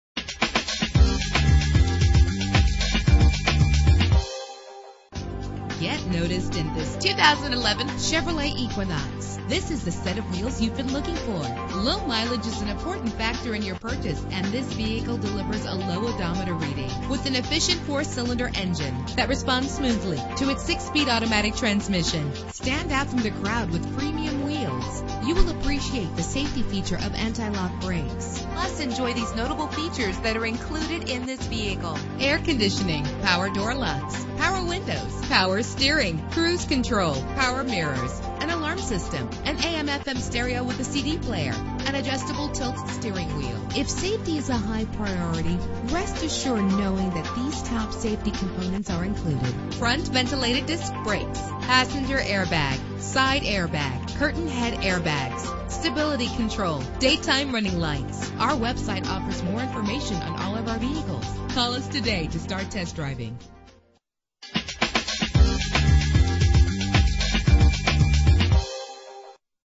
2011 Chevrolet Equinox - Homosassa FL
Year: 2011
Make: Chevrolet
Model: Equinox
Trim: LS
Engine: 2.4 liter inline 4 cylinder 16 valve
Transmission: 6-Speed Automatic
Color: Gray
Mileage: 7372
Address:
1275 S Suncoast Blvd